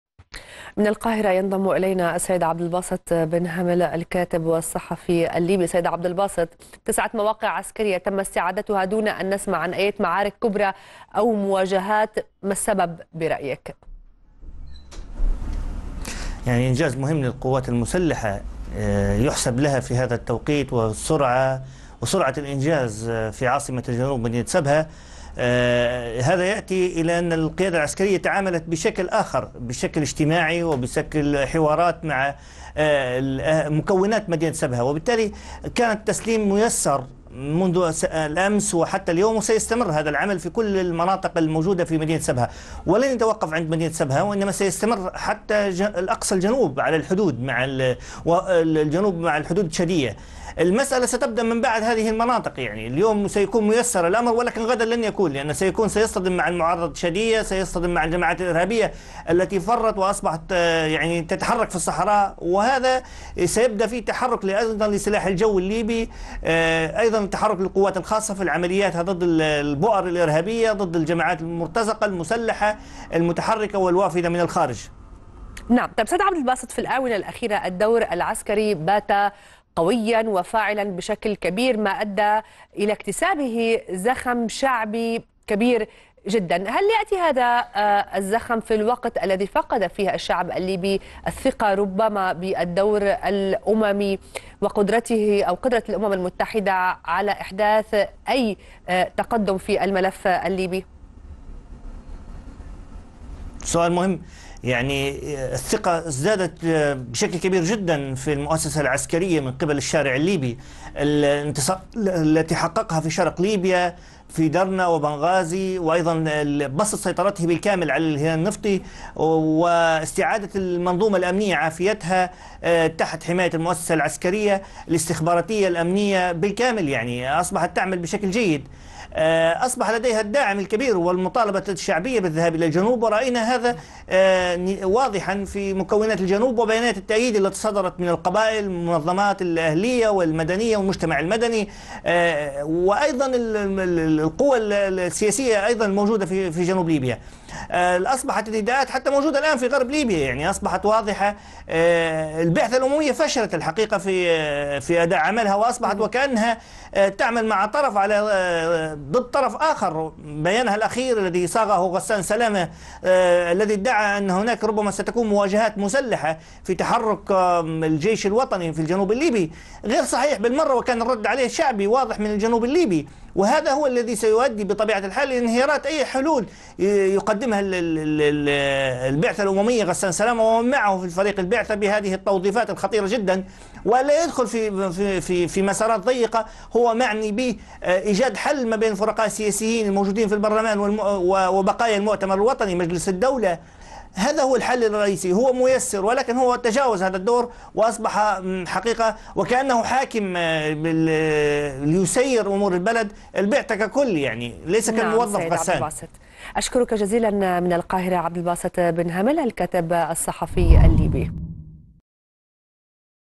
محلل: استراتيجية الجيش الليبي نجحت في السيطرة على عدة مواقع بـ "سبها"
أعلن الجيش الوطني الليبي استعادة 9 مواقع عسكرية بمدينة سبها جنوب البلاد ، بعد هجمات جديدة استهدفت أوكار الجماعات المسلحة، ضمن ما أطلق عليها عمليات تحرير الجنوب الليبي.
وأكد بيان للجيش الليبي أنه جرى تسليم هذه المواقع بالكامل إلى قوات الجيش طواعية، بالتعاون مع أعيان مدينة سبها.
المزيد من التفاصيل يتحدث المحلل السياسي عبد الباسط بن هامل، مع الإعلامية يارا حمدوش عبر النشرة الإخبارية لقناة الغد.